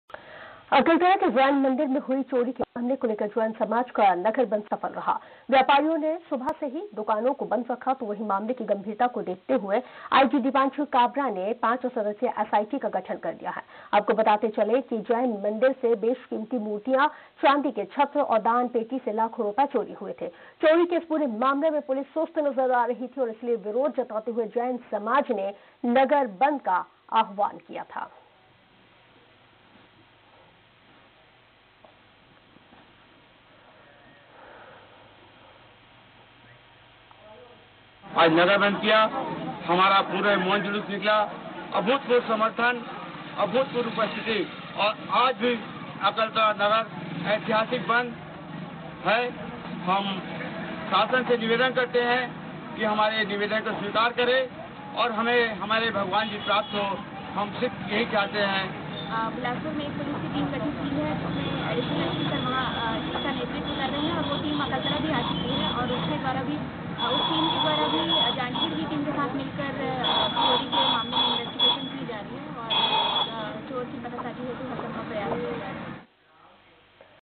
Janjgir Champa News: जैन मंदिर से हुई मूर्तियों चोरी मामले में जैन समाज द्वारा किया नगर बंद
Janjgir, Chhattīsgarh, India Janjgir-Champa Police जैन_मंदिर Jain samaj